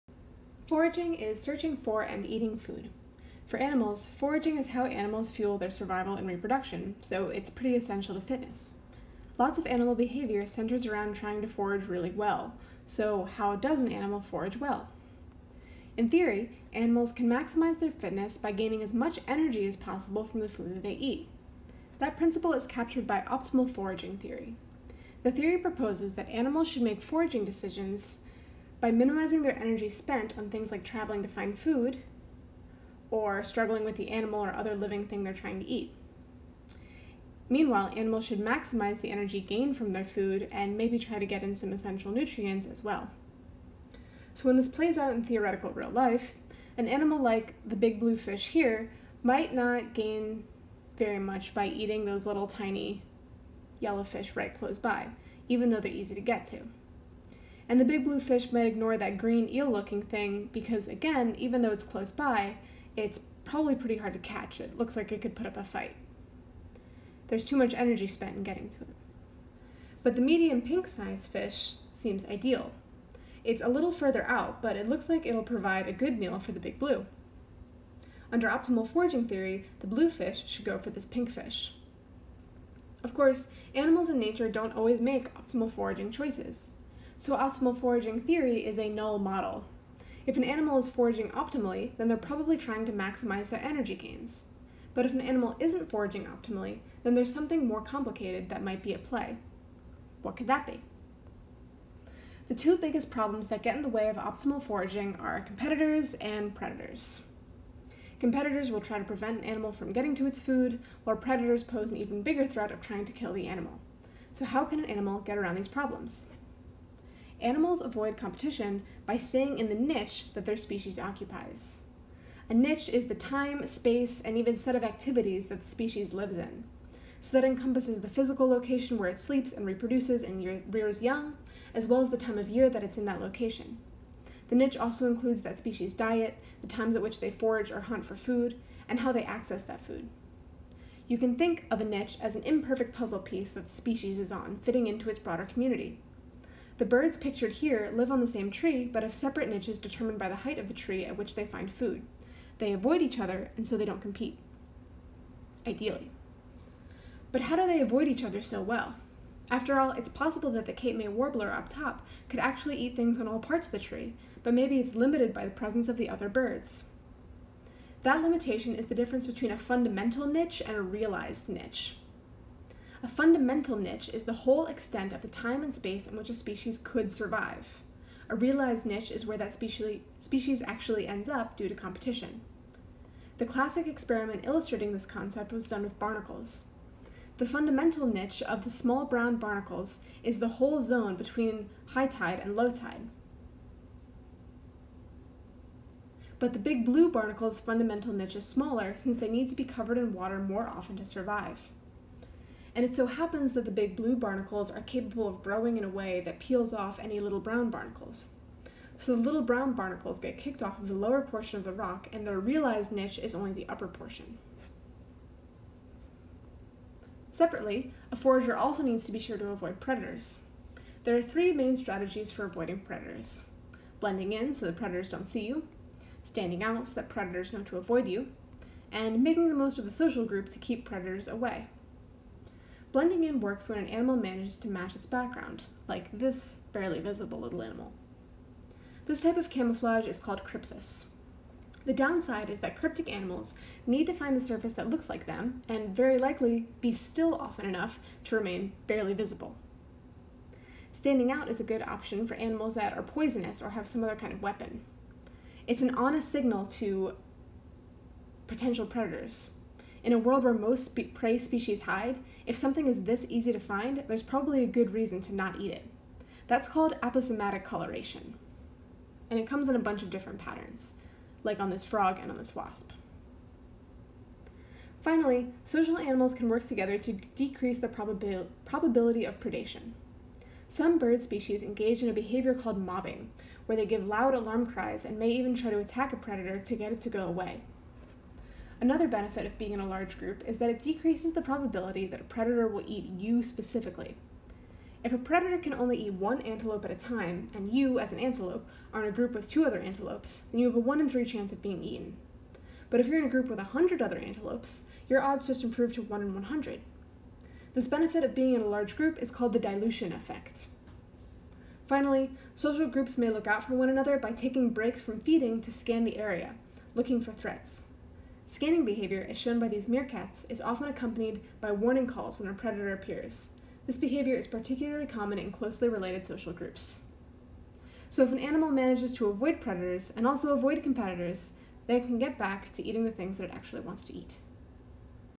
Foraging
Full transcript in comments

Loosely based on Chapter 6 of Alcock and Rubenstein’s Animal Behavior textbook.